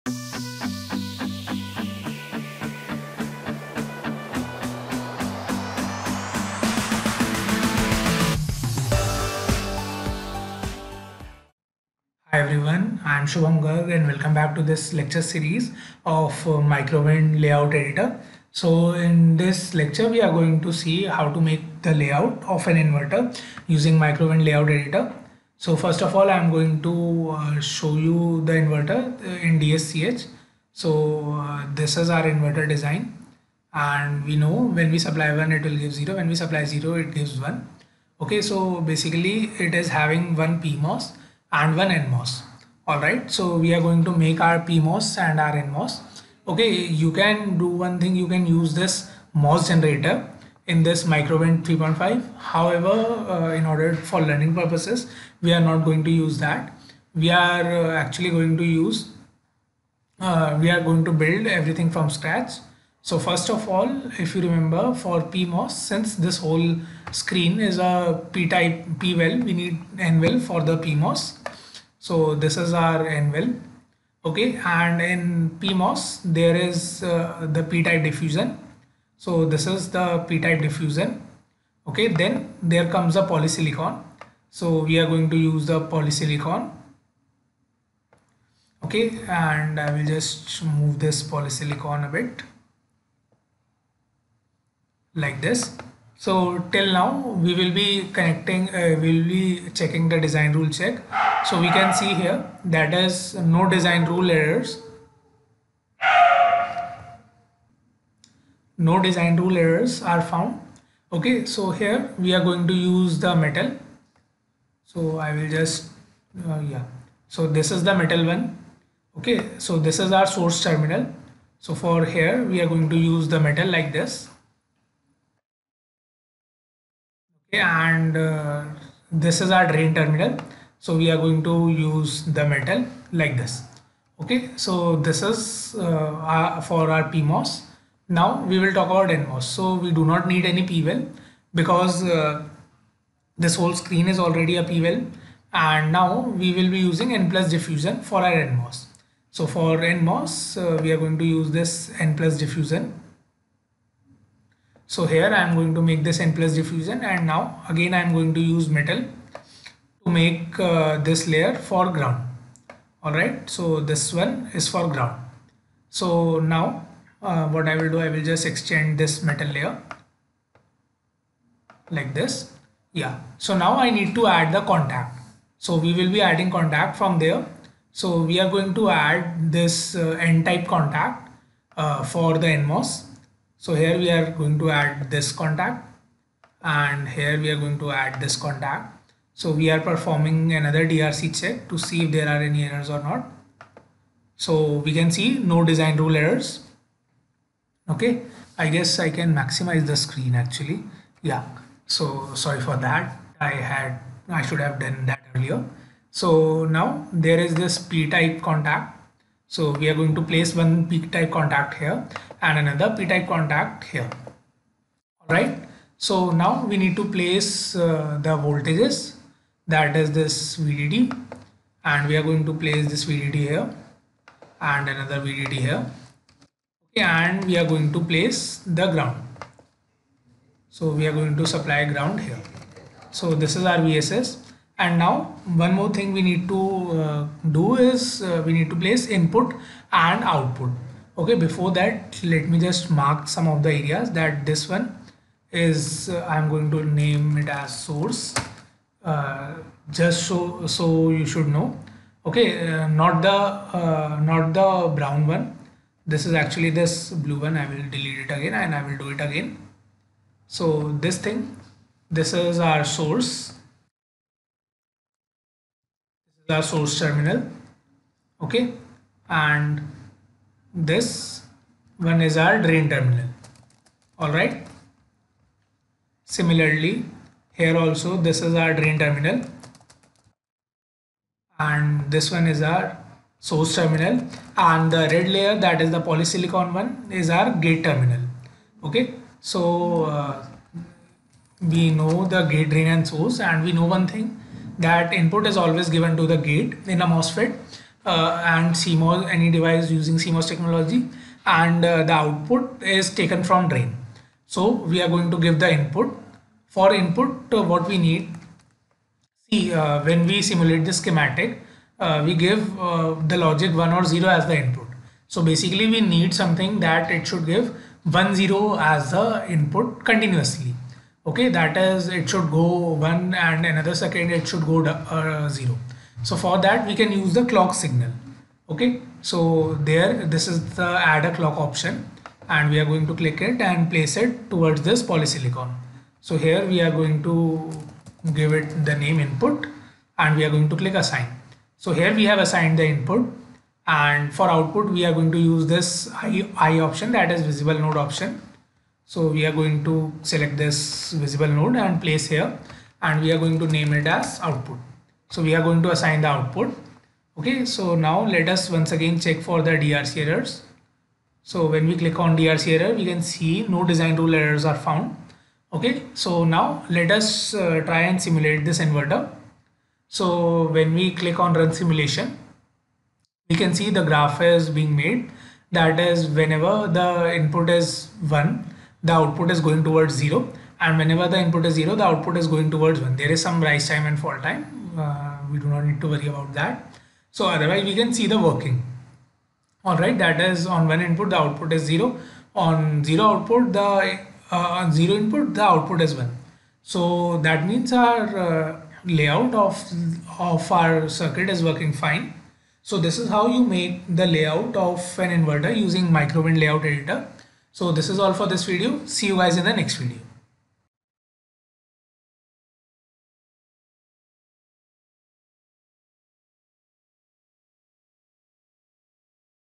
Inverter Layout using Microwind Layout Editor | L-Edit
In this Video we are going to create the layout of an inverter using Microwind L-Edit. We are also going to perform DRC Check. Lastly we will simulate the layout to obtain the waveform to verify the working of the circuit.